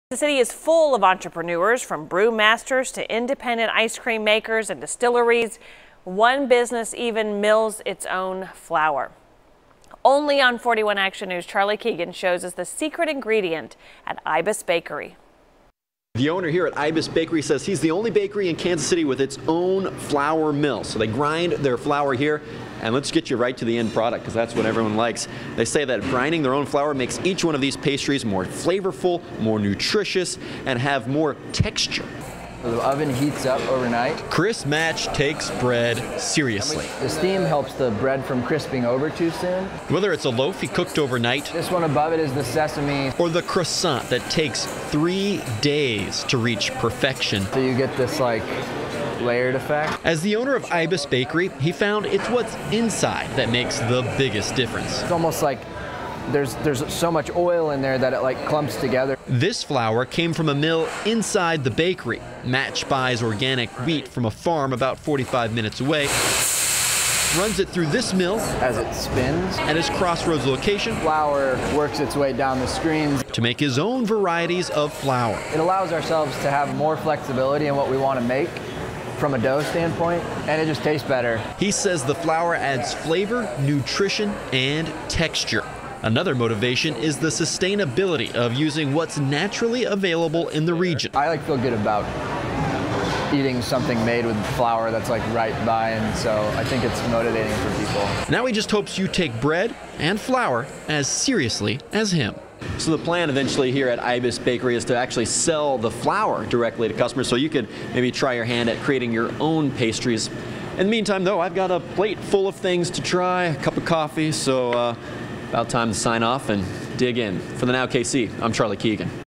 Flour power: Kansas City bakery mills its own flour
Ibis Bakery's owner said fresh flour adds flavor, nutrition and texture to the finished products.